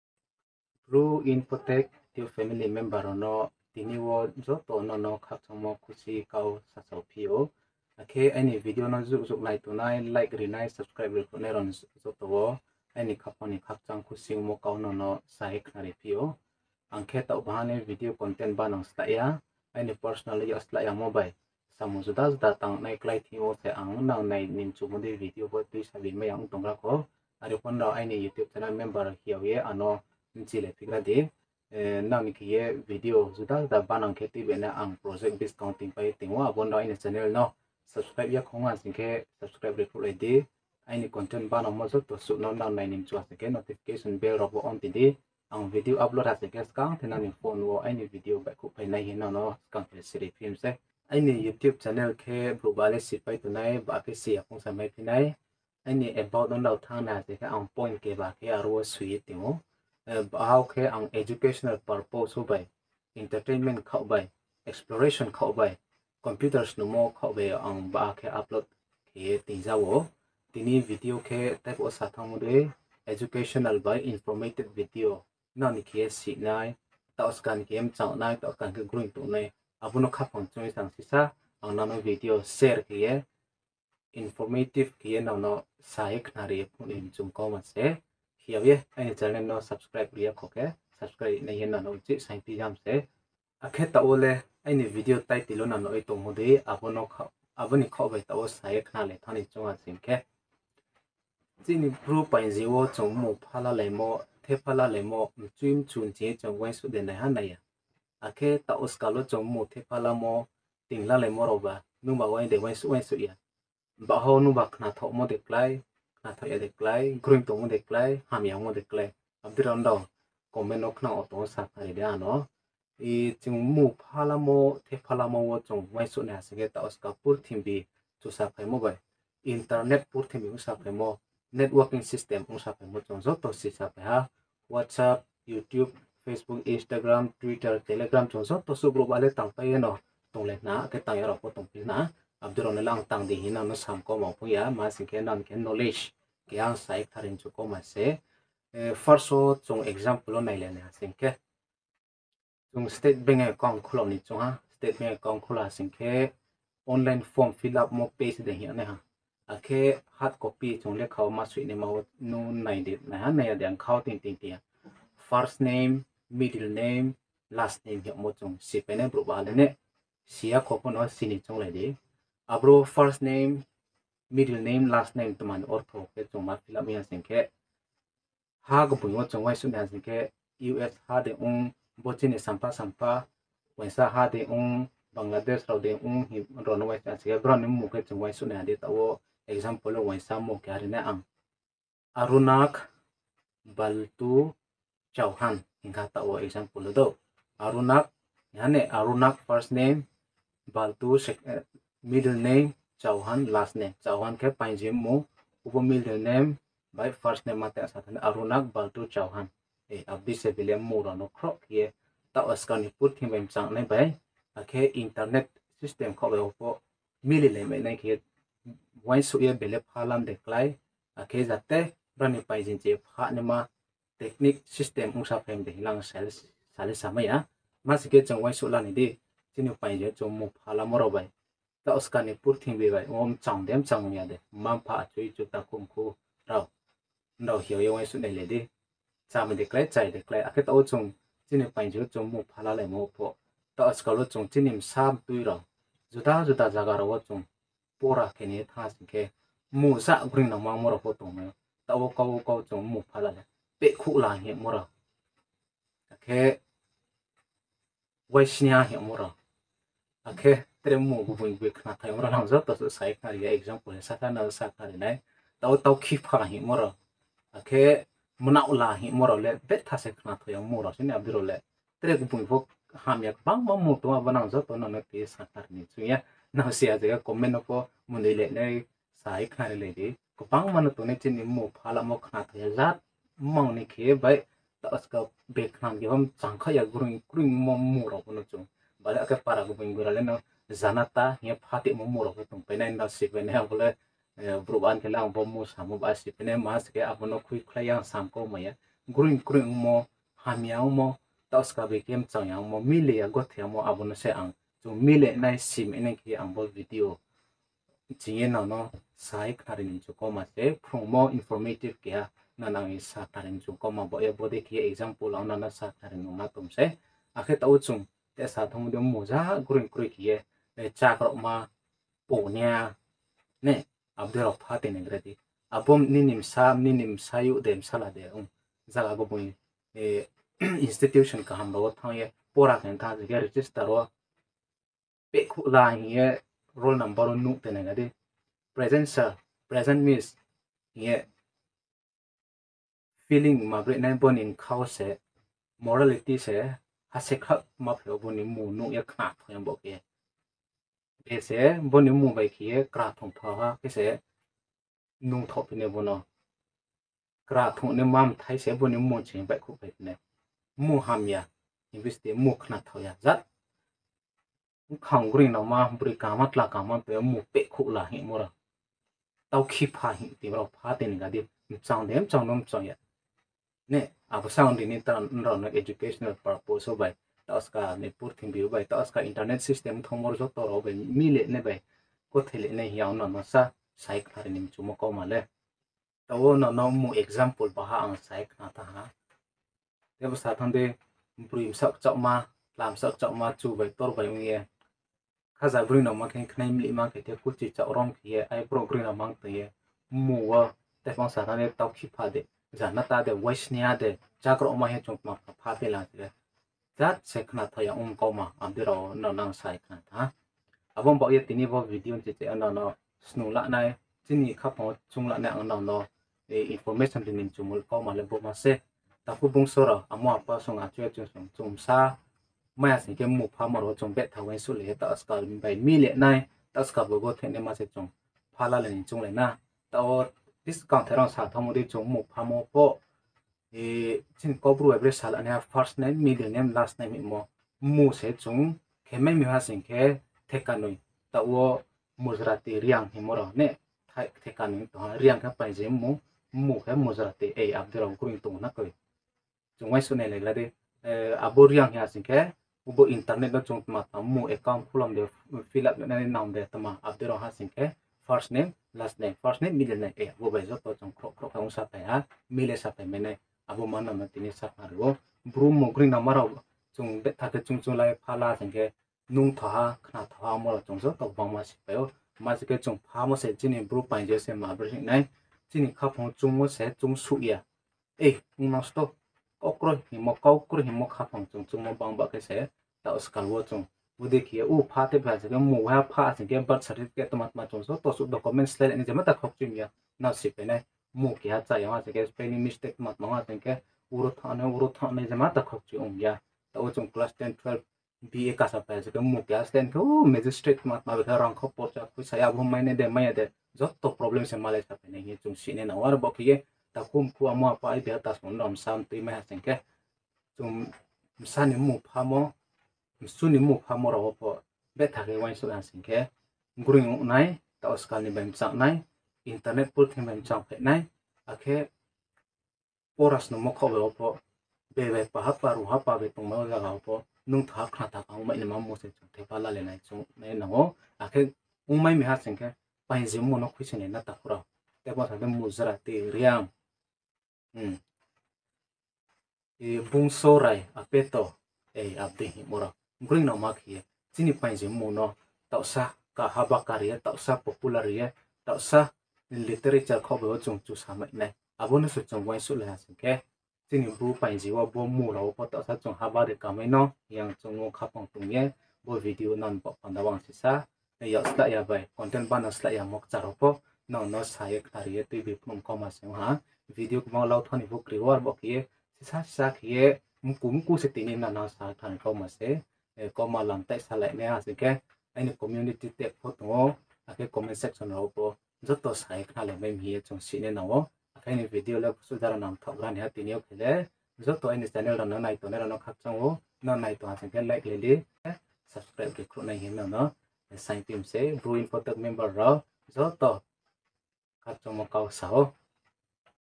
BRU Paingji Bai Mmung Pha La Mo | BRU Informative Video
Bo video wo khe chini BRU Paingji wo mmung pha lalai mo wo tauhsa gruing tung mai khe hingmo khaphang ma chung thingyo ang informative video banang ja wo. Ada le cha yakya pho ungmai wo, are phono betha waingsuh lai ye chini msa mtui msu mthai ni mmung pha lai mo wo tauhsa kaham waingsuh ye pha lai mai khe ham ja nai hna. Akhe paingji/ jati no chini mmung pha mo wo kbeh sing ri yaknai chunglai sra sna.

BRU INFOTECH